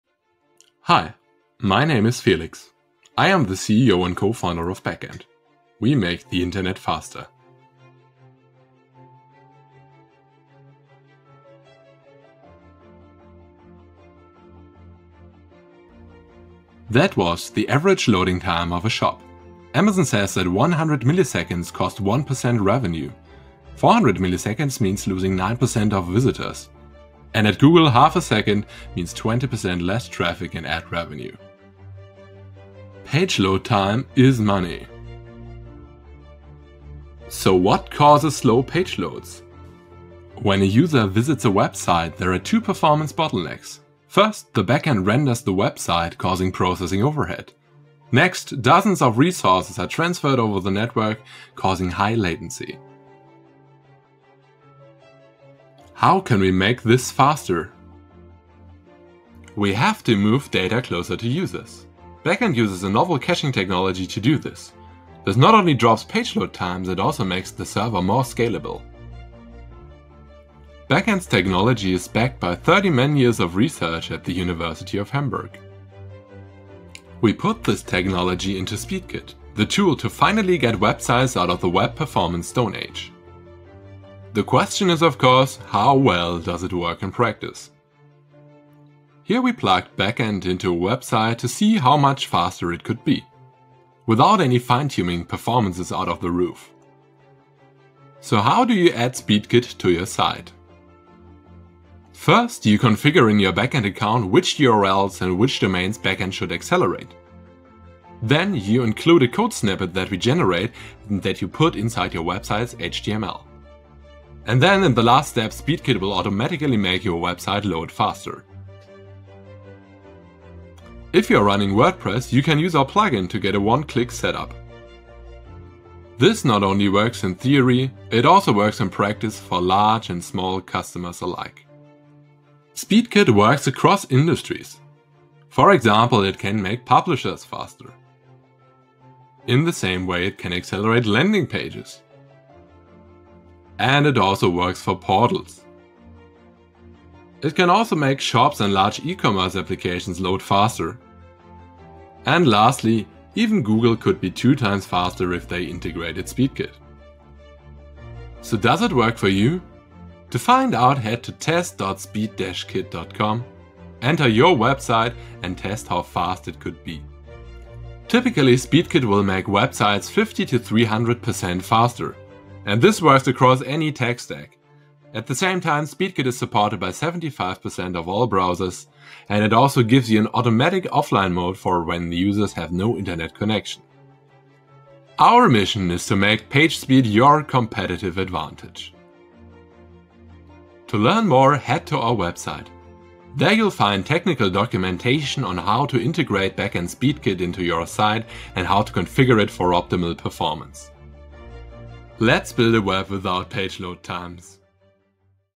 Speed Kit: Getting Websites out of the Web Performance Stone Age
Page load time is money. This is not only true for companies like Amazon that lose more than $1.3B in revenue per year if their website is a 10th of a second slower. It is also true for publishers, whose business model depends on a user experience that facilitates consumption of as much content as possible. However, many publishers have heterogeneous and complex technology stacks that make it extremely hard to tackle performance, scalability and page load time.

Novel browser technologies now offer a means of making web performance as simple as including a script. The research spin-off Baqend has developed a "Speed Kit" that directly hooks into existing publisher, e-commerce and brand website and makes them 50-300% faster. In this pitch, we will explain how the technology works for large websites.